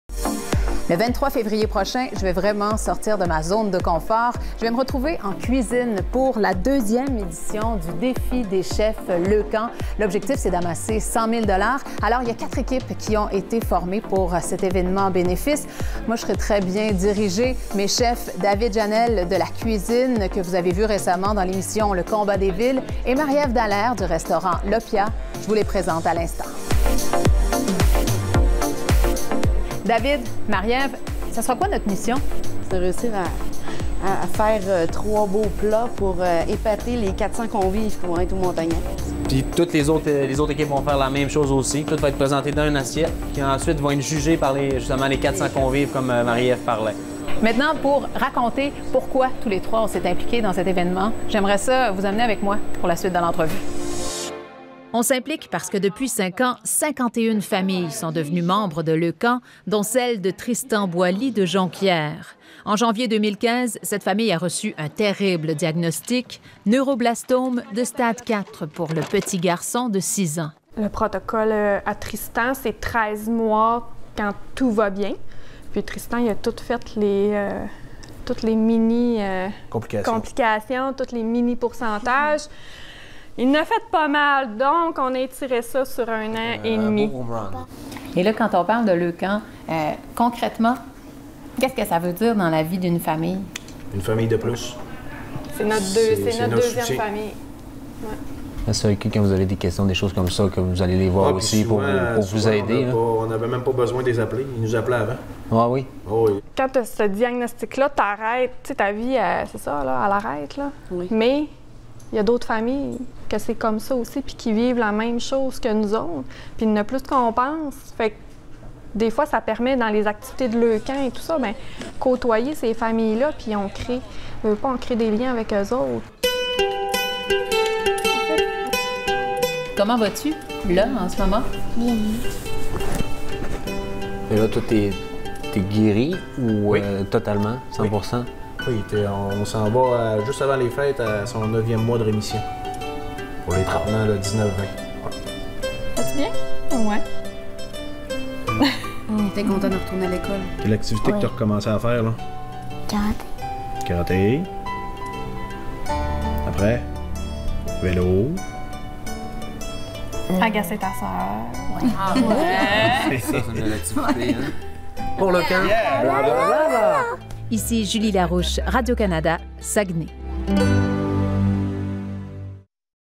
Défi des Chefs Leucan 2017
Le deuxième Défi des Chefs Leucan 2017 Saguenay—Lac-Saint-Jean se tiendra en février. Cette compétition culinaire est une activité bénéfice très importante pour l'organisme. Julie Larouche vivra l'expérience en cuisine avec les chefs Chef David Janelle du Restaurant La Cuisine et Marie-Eve Dallaire du Restaurant Opia.